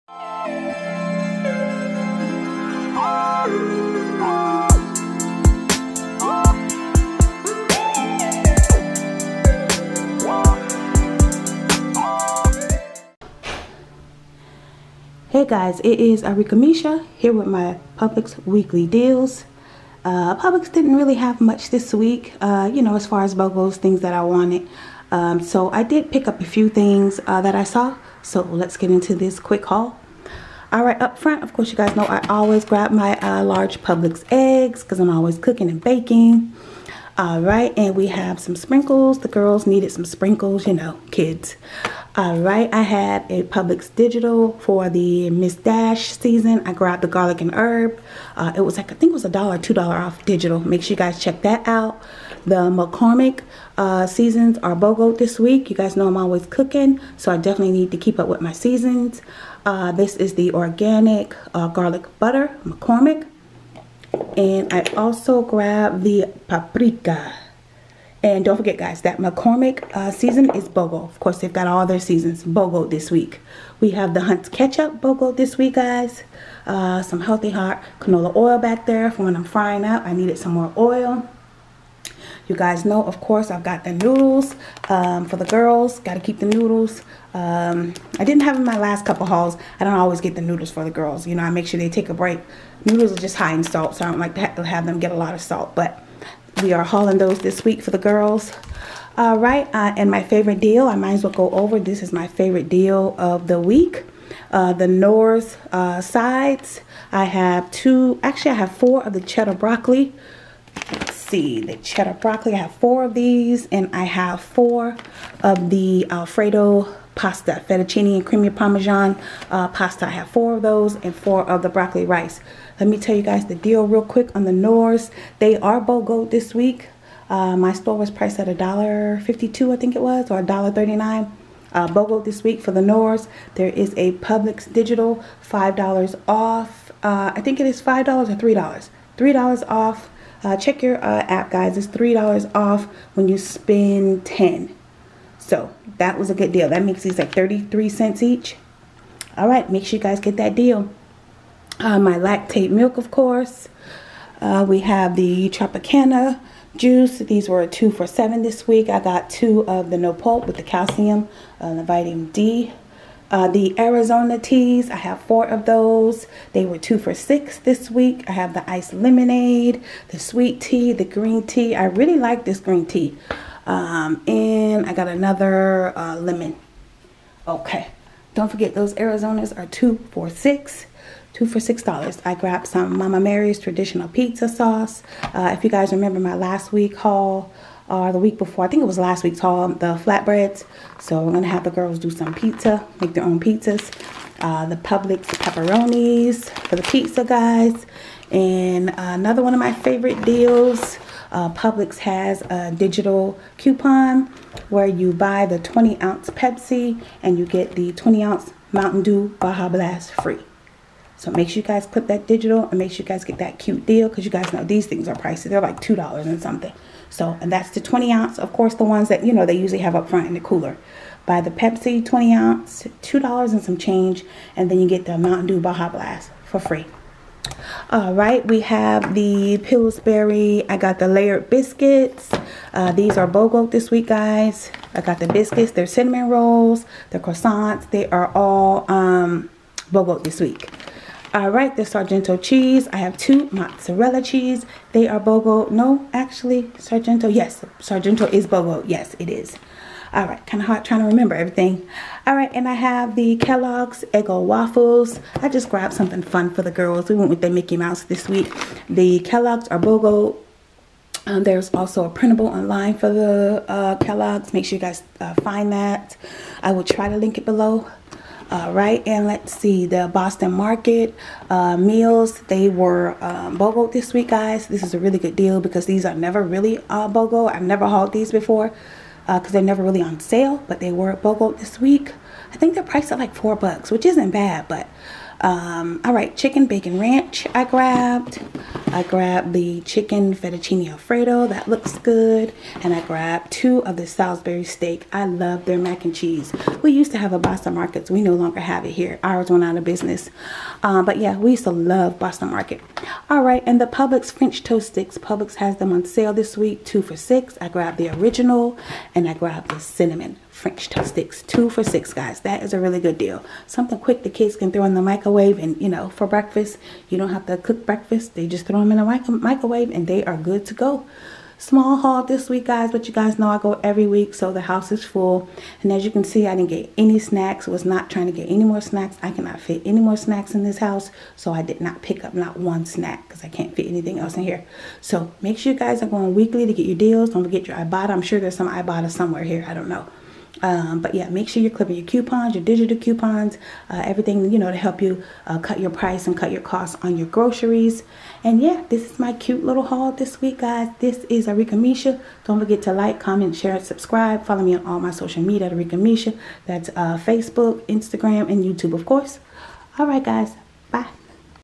Publix Grocery Haul/2-15-24 to 2-21-24/BOSTON MARKET MEALS BOGO/
These are my deals for the week. Publix did not have that many Bogo deals this week.